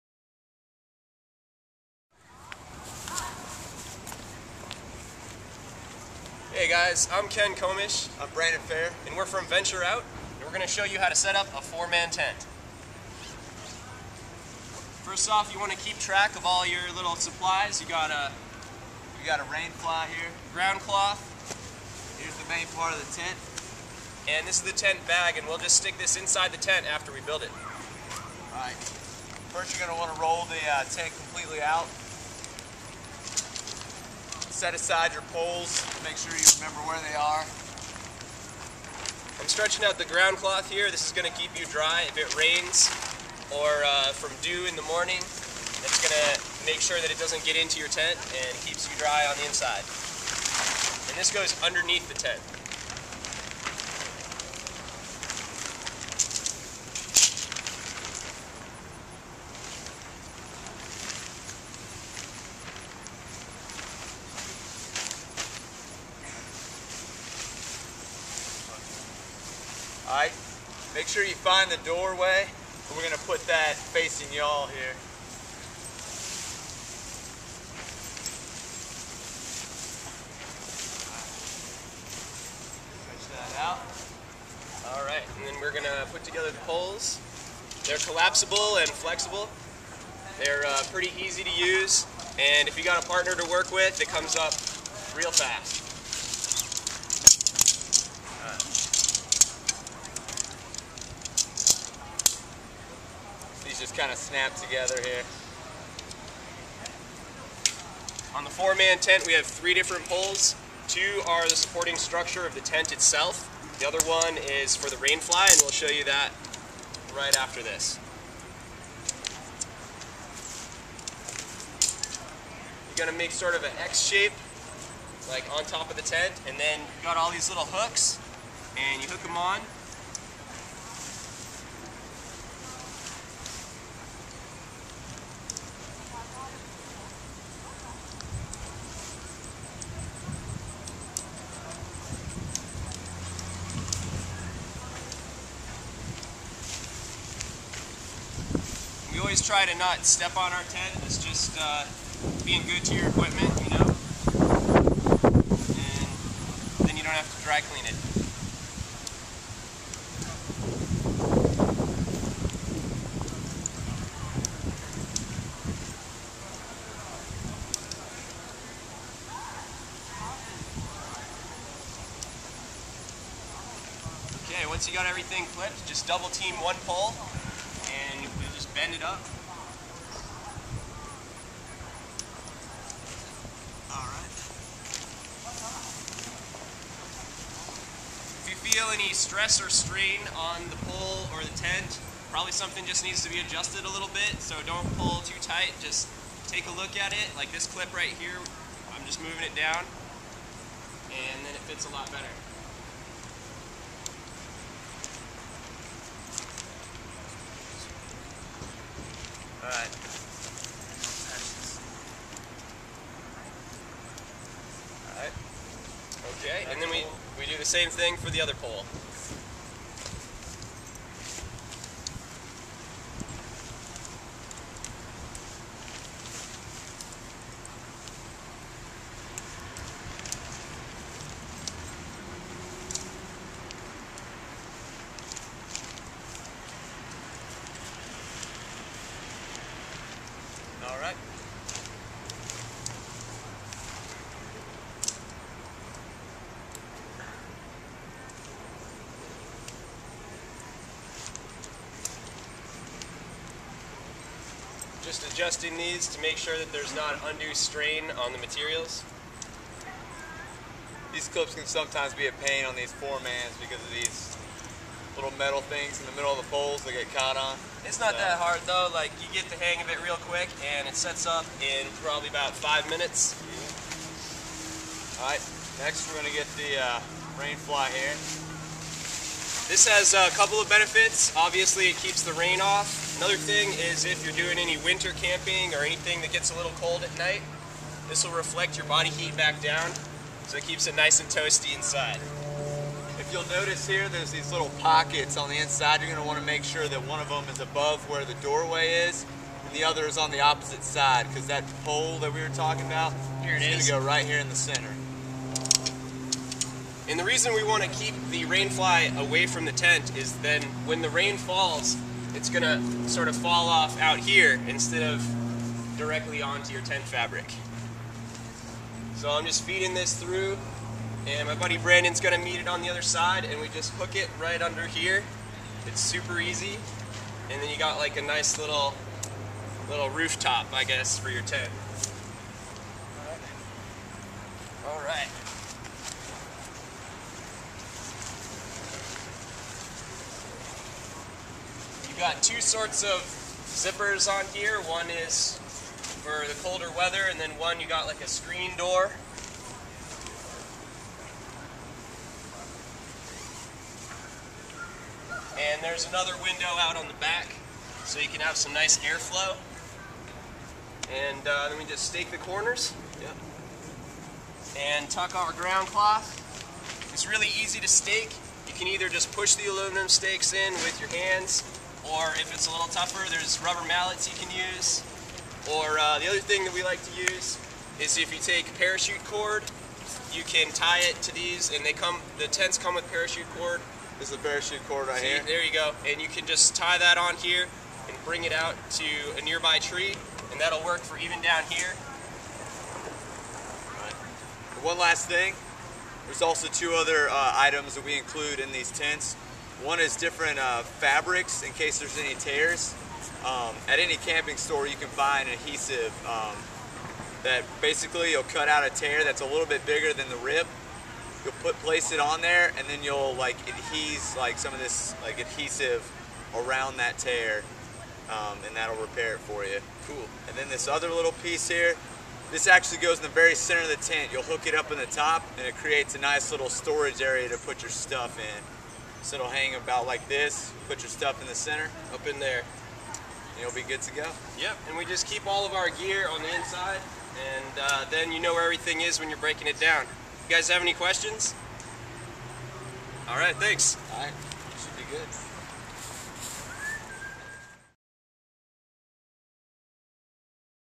How to set up a 4 man tent
Venture Out staff demonstrate the basics of setting up a tent